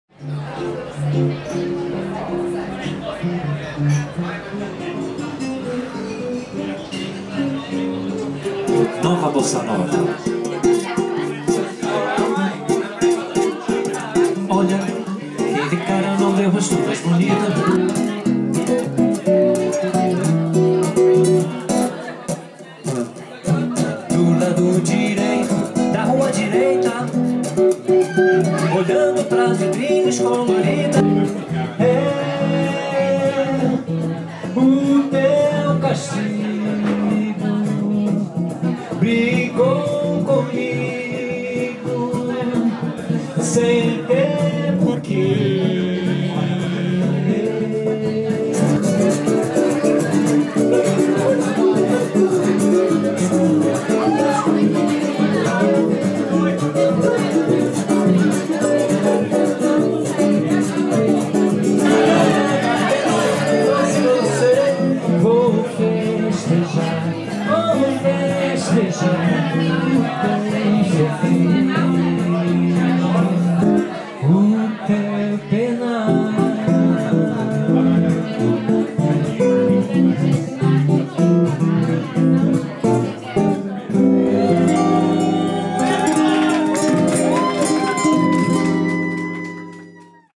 Bossa Nova Live @ In Parma by Food Roots 20-03-12
Gui Tavares, Brazilian, played for the first time at In Parma, Fitzrovia, London.